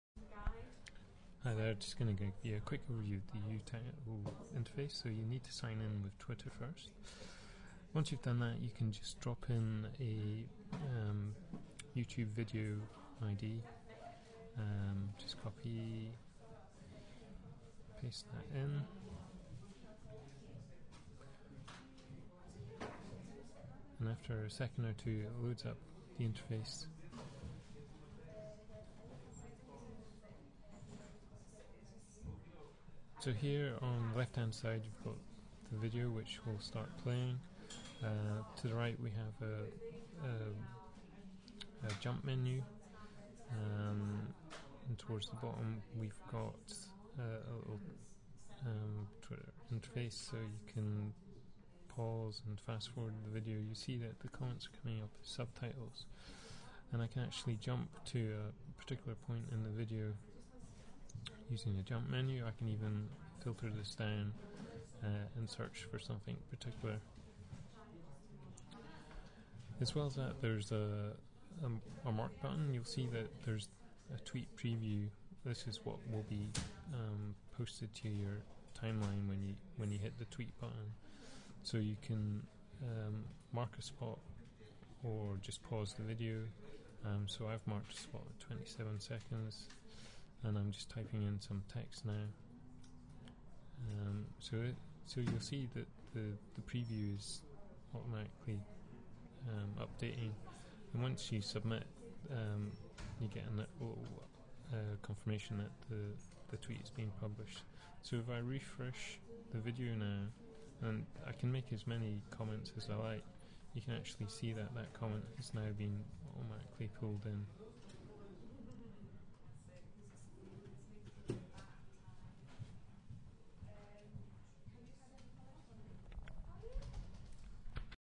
Overview of uTitle interface (timeline based comments of YouTube videos using twitter)
Demonstration of the uTitle interface which allows timeline commenting of YouTube videos using Twitter.